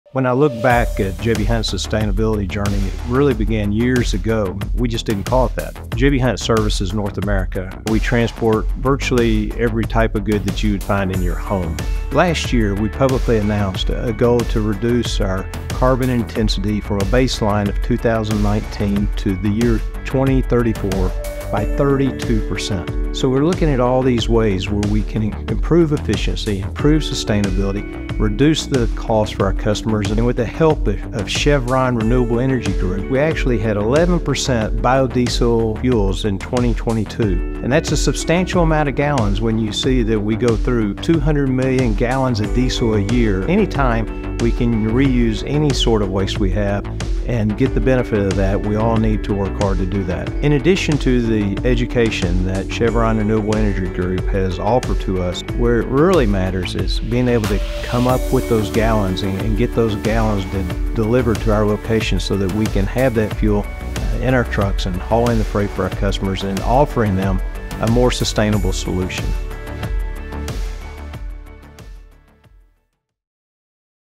J.B. Hunt's Craig Harper on what its like to work with Chevron Renewable Energy Group
It all started with the vision of a young couple from Arkansas — Johnnie Bryan (J.B.) Hunt, and his wife, Johnelle Hunt. Starting with just five trucks, under the Hunts’ leadership, the company grew rapidly over the next several decades, ultimately becoming a publicly traded company in 1983, adding intermodal service in 1989 and dedicated transportation service in 1993.

It was around this time that Craig Harper, who now serves as Chief Sustainability Officer (CSO) for J.B. Hunt, joined the company. For as much change and growth that happened prior to his arrival, he’s witnessed and contributed extensively to just as much change in the last 31 years since his arrival.

Having served as Chief Operating Officer for the bulk of his time at the company, in recent years, his focus has shifted toward advancing the company’s sustainability efforts as CSO. While that job comes with a new title, the initiatives to be more sustainable and efficient are certainly nothing new to J.B. Hunt.